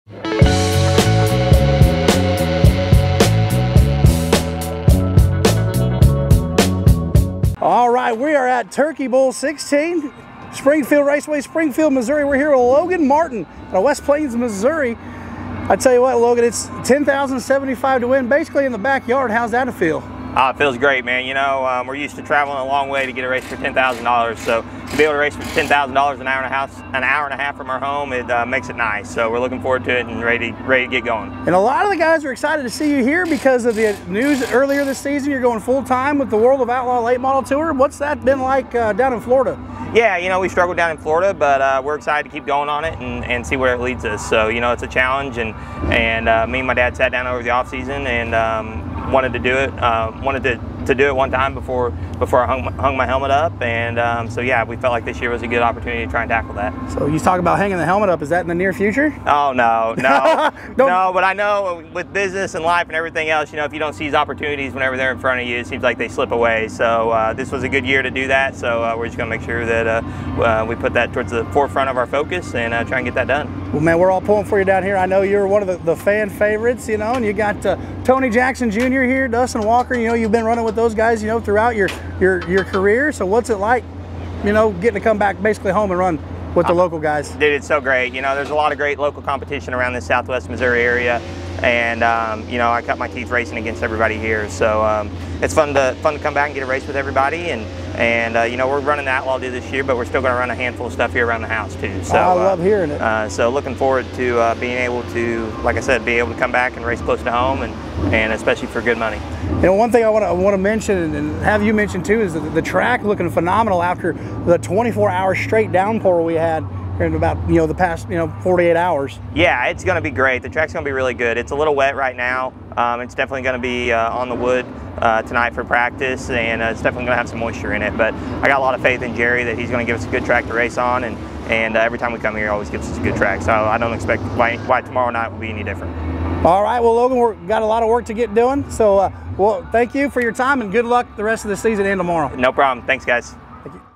Logan Martin Interview at Springfield Raceways Busch Light Turkey Bowl XVI
Exclusive interview with Logan Martin as Trenton Blackburn talks to him about Busch light Turkey Bowl XVI.